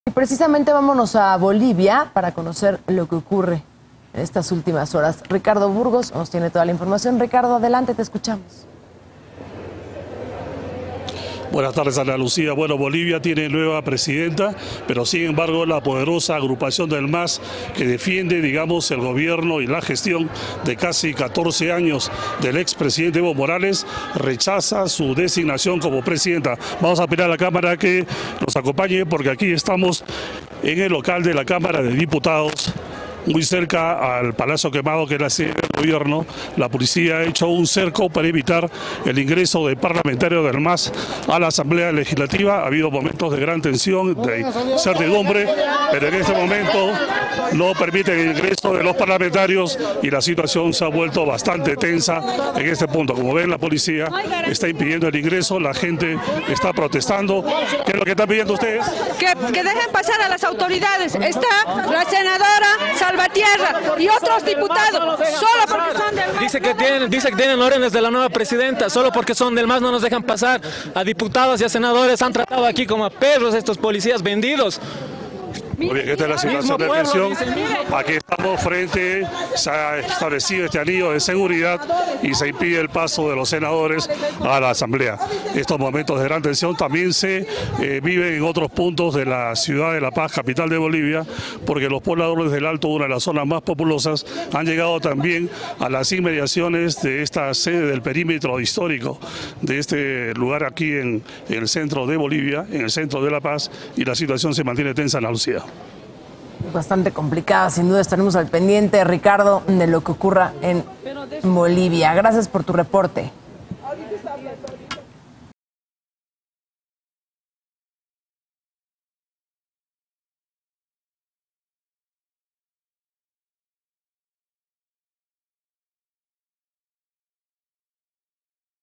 Policías resguardan Asamblea de Bolivia, en La Paz - Paralelo 23
Policías resguardan Asamblea de Bolivia, en La Paz; legisladores del MAS denuncian que les han negado la entrada